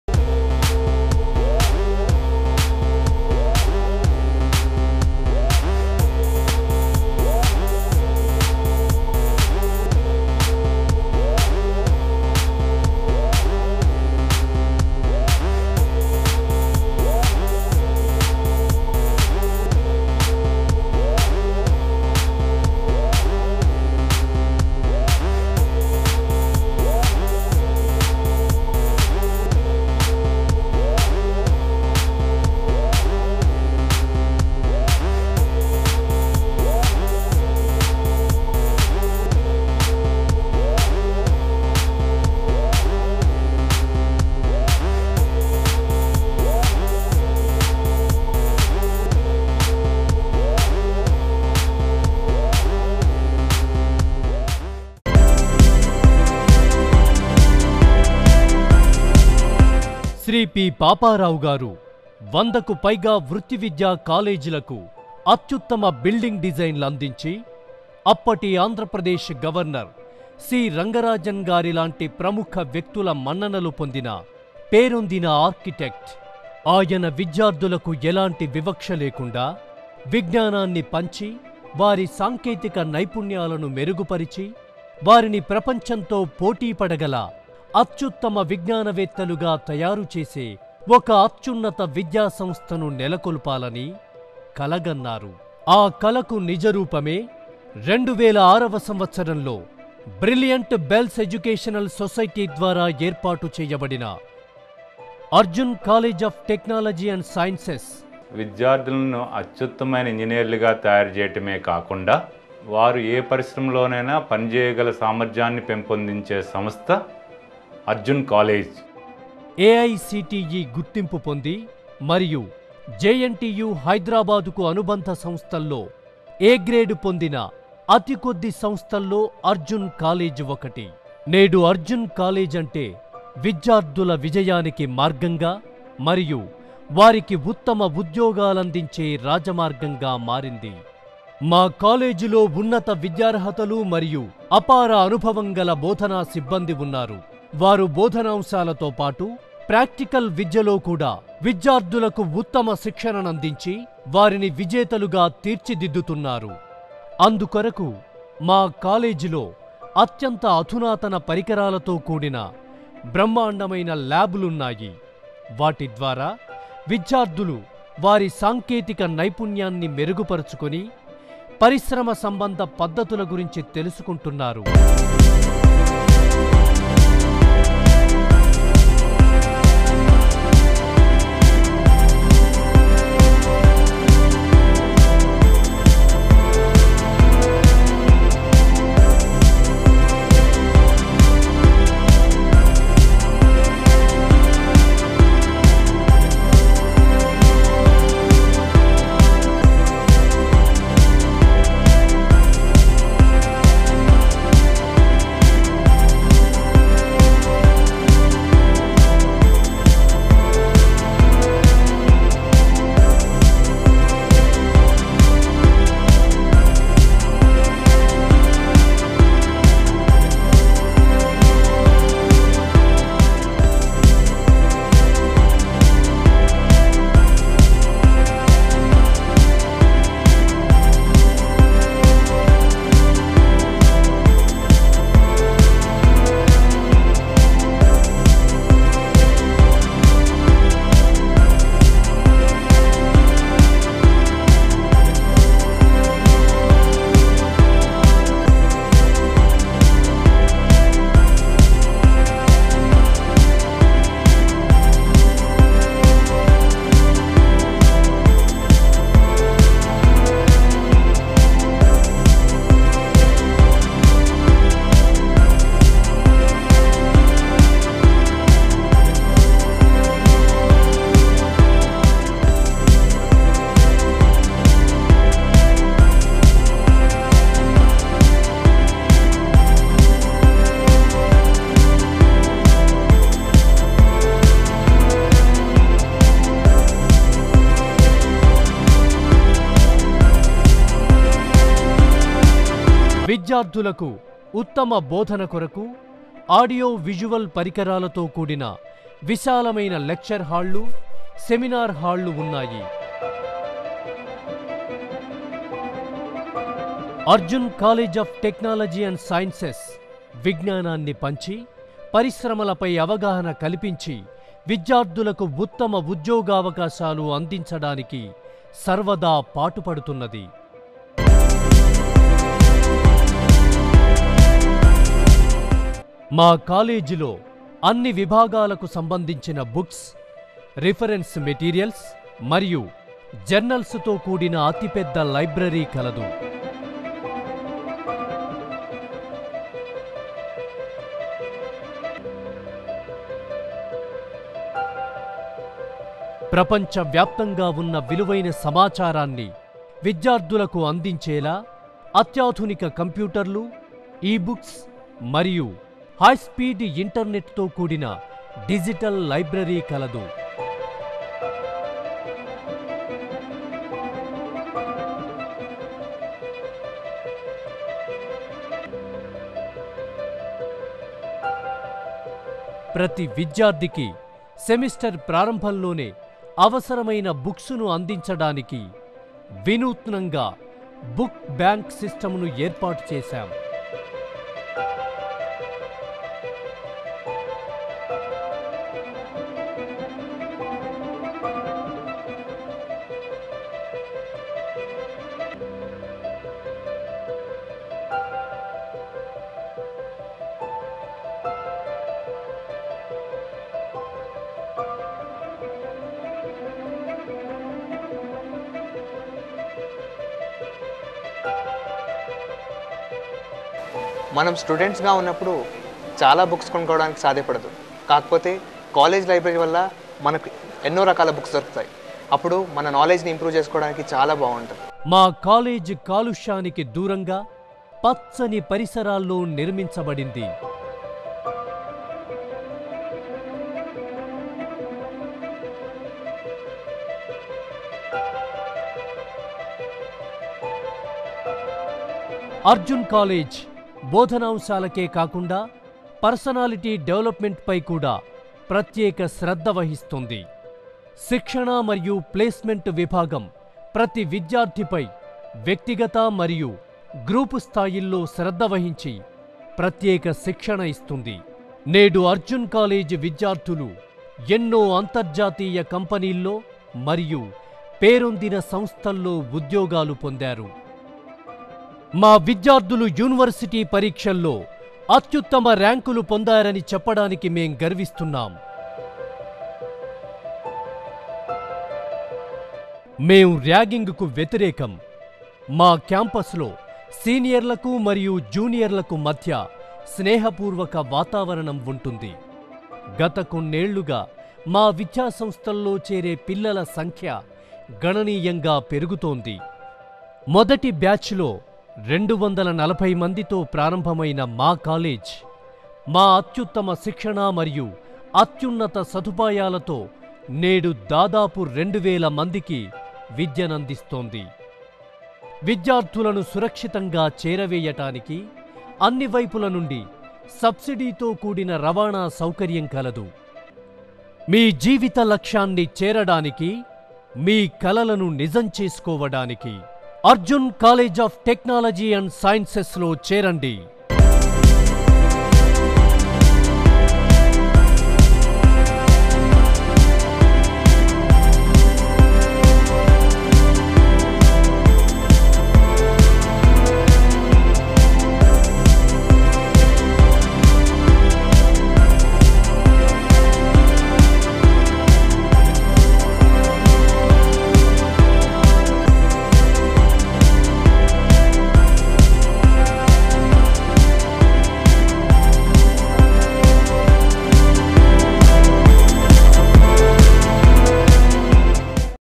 Arjun College of Technology & Sciences-Telugu
This film features about Arjun College of Technology & Sciences, Mount Opera, Greater Hyderabad. This film is made in 2012.
Script & Direction: Y. Lenin Babu (YKY Solutions)